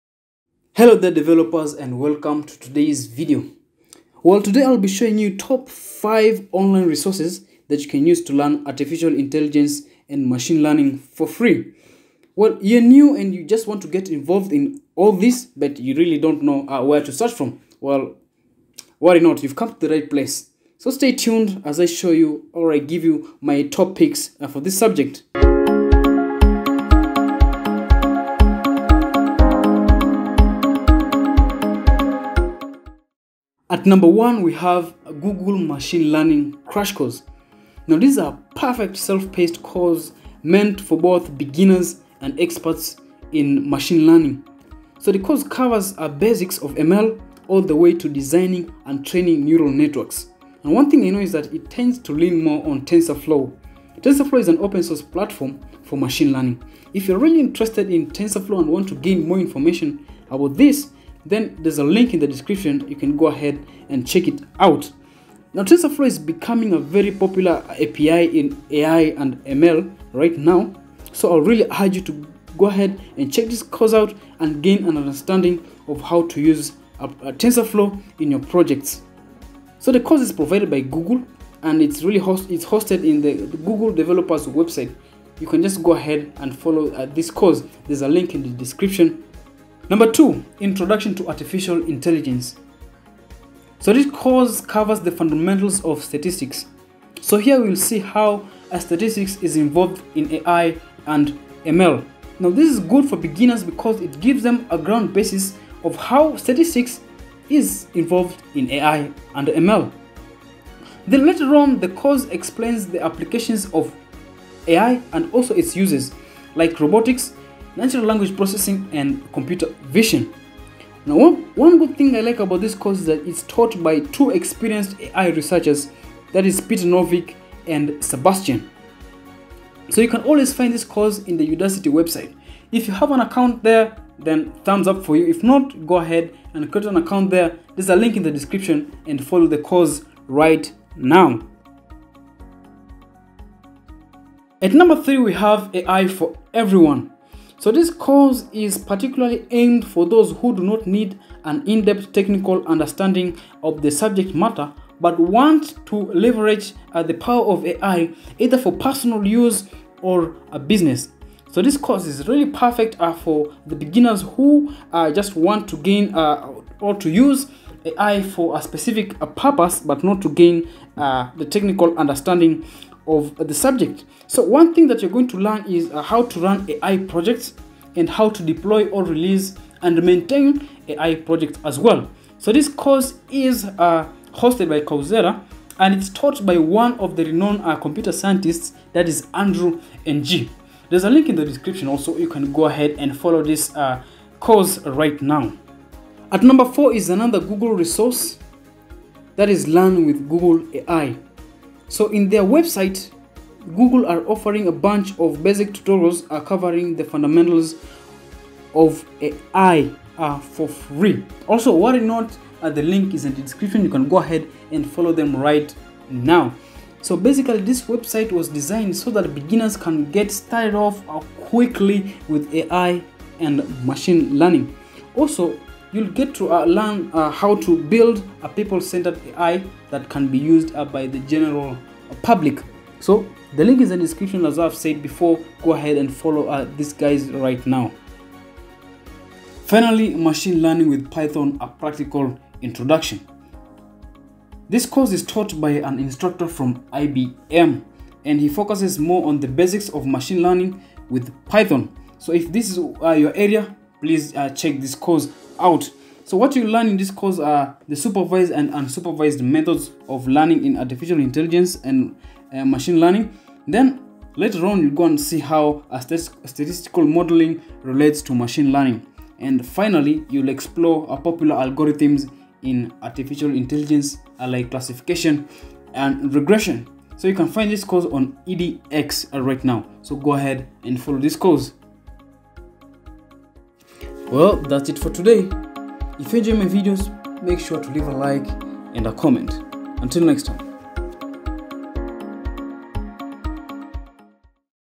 TOP 5 FREE AI AND ML COURSES FOR BEGINNERS (2020)
Getting started in AI and ML is a bit tedious especially for beginners. There's so many sources out there yet it is still difficult to find one that suits you best.Here are my best picks for Artificial Intelligence and Machine Learning courses to jump start your career into AI and ML.

1. Google Machine Learning Crash Course

2. Introduction to Artificial Intelligence

3. Artificial Intelligence For Everyone

4. Learn With Google AI

5. Machine Learning With Python